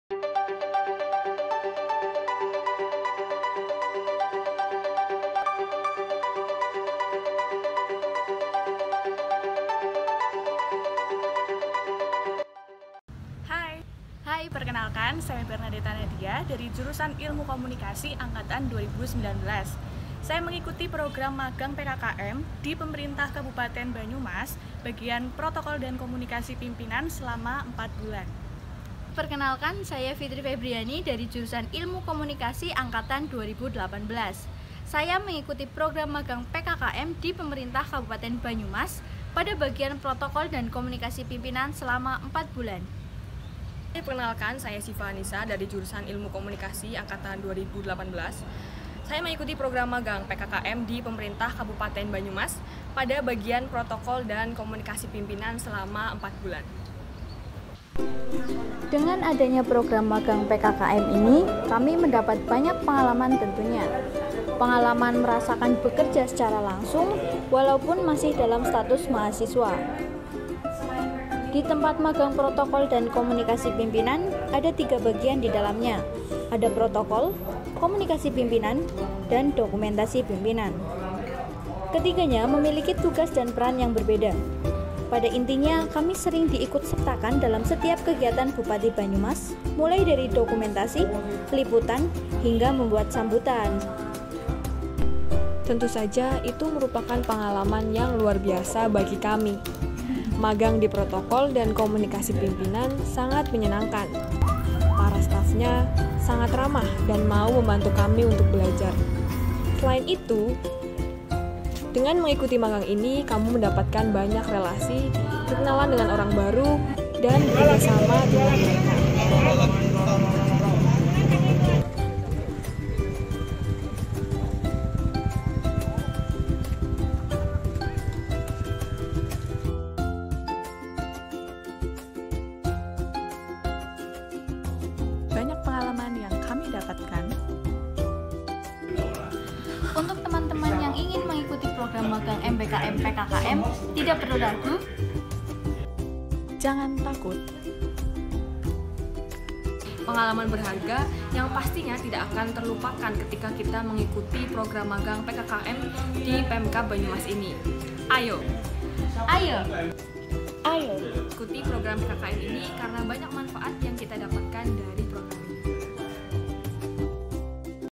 SIMAK! TESTIMONI PESERTA MAGANG MBKM DI PEMKAB BANYUMAS
Kesan dan pengalaman mereka yang magang MBKM dari Prodi Ilmu Komunikasi FISIP Unsoed di Humas Pemkab Banyumas